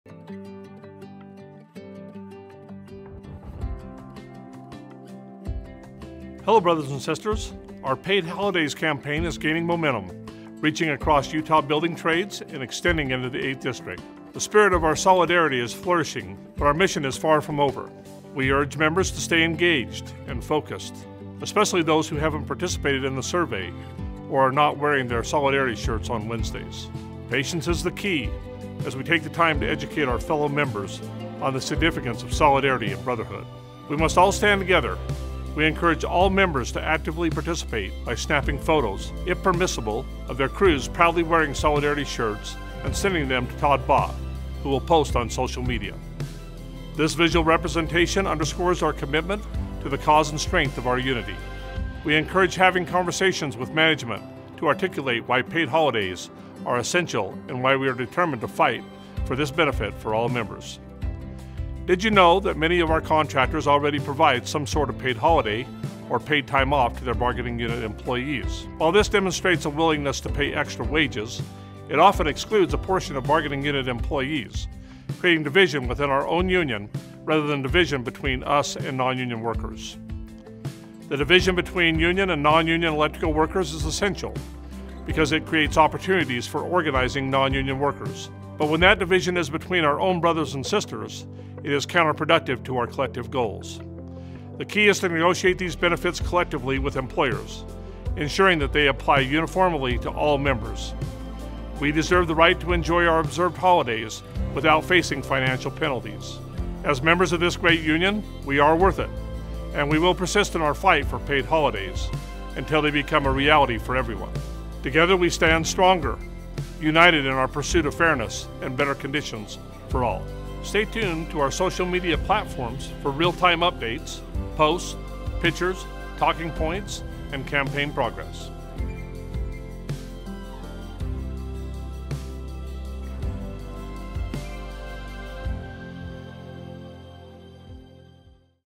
IBEW 354 Business Manager's Minute: Support Our Holiday Campaign!
Join the movement - our Paid Holidays Campaign is gaining momentum in Utah's building trades and beyond, extending into the 8th district! While the spirit of solidarity flourishes, our mission is ongoing. We urge all Local 354 members to stay engaged!

Stand together by proudly wearing your red shirts every Wednesday. Snap pictures of your crew, if permissible, in solidarity shirts - let's flood social media with our strength. We deserve to enjoy our observed holidays without financial penalties and will persist until paid holidays are a reality for all. Follow us for updates and join the fight for fairness! Together we stand stronger, united in our pursuit for fairness and better conditions for all.